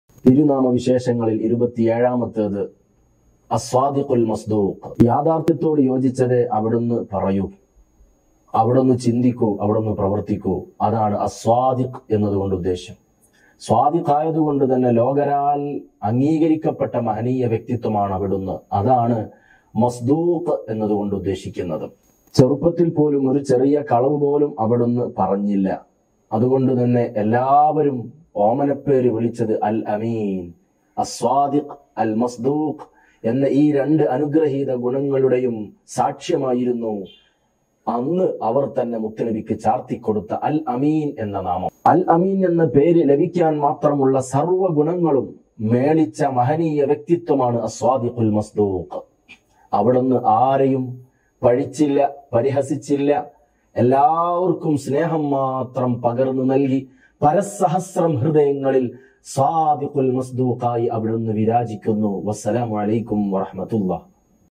തേൻ തുള്ളികൾ | റബീഉൽ: അവ്വൽ 27
റബീഉൽ: അവ്വൽ 27
തേൻ തുള്ളികൾ
തിരുനാമങ്ങളിലൂടെ ഒരു തീർത്ഥയാത്ര
ഹാഫിള് മുഹമ്മദ് ശരീഫ് സഖാഫികൊളത്തൂർ